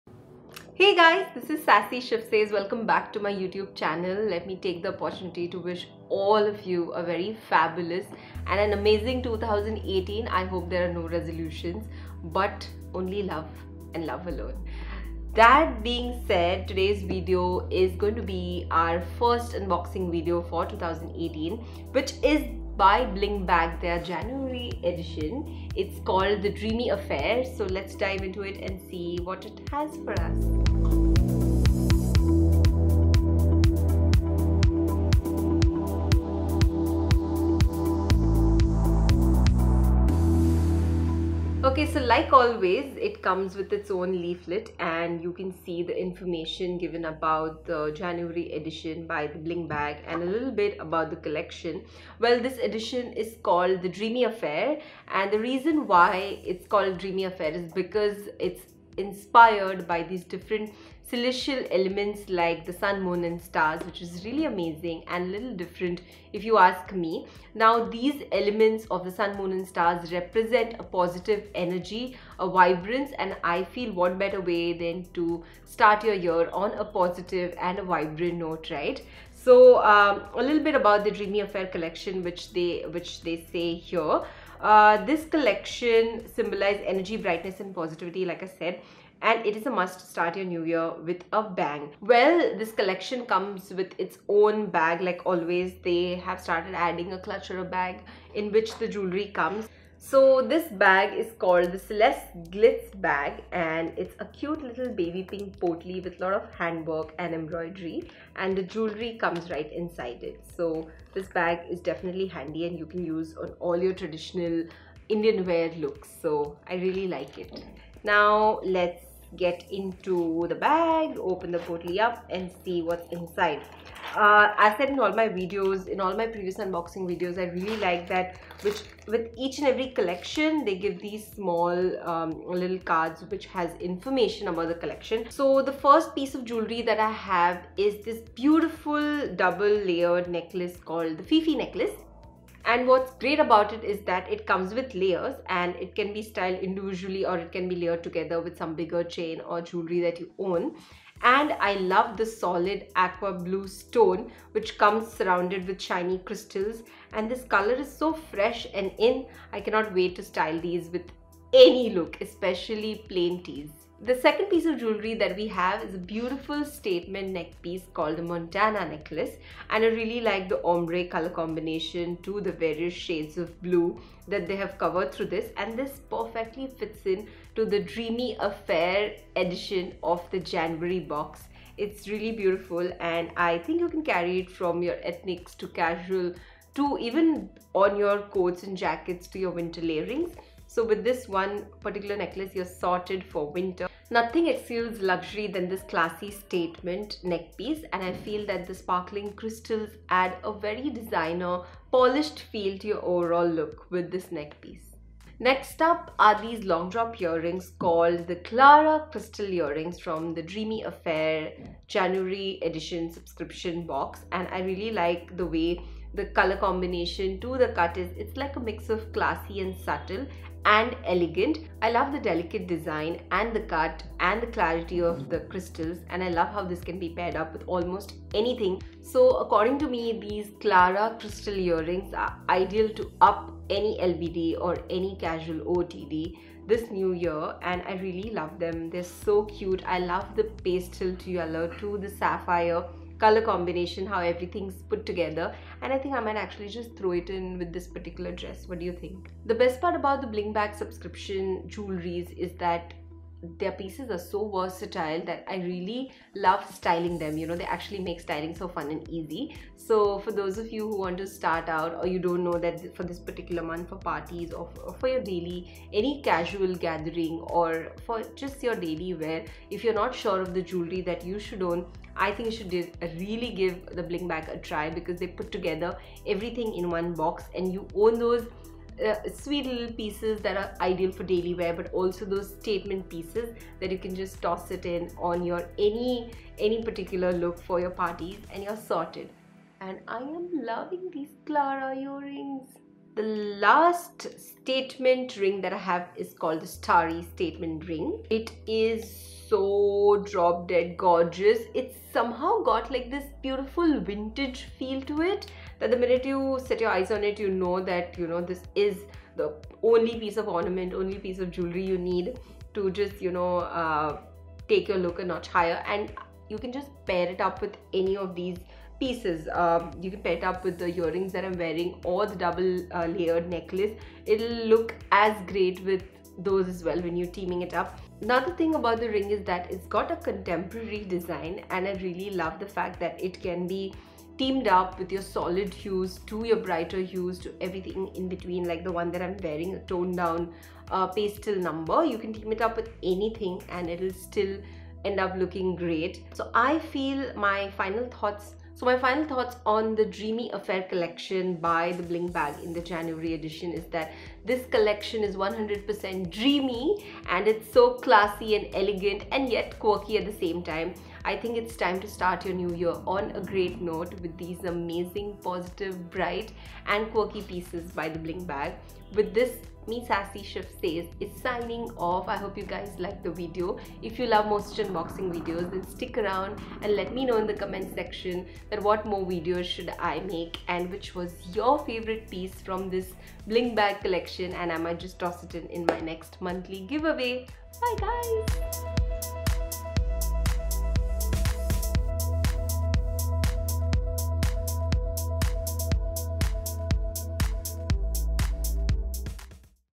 What's in my Bling Bag January 2018 Edition
The January Bling Bag Edition is indeed a dreamy affair. Find out which celestial stones are brightening up my New Year and if these pieces should be on your Must have radar. The Bling Bag is a unique monthly jewelry Subscription box concept which brings forth some interesting pieces and baubles to us as per the latest trends.

Copyright:-
Everything you see in this video was created by me (Shifa Merchant) unless otherwise stated.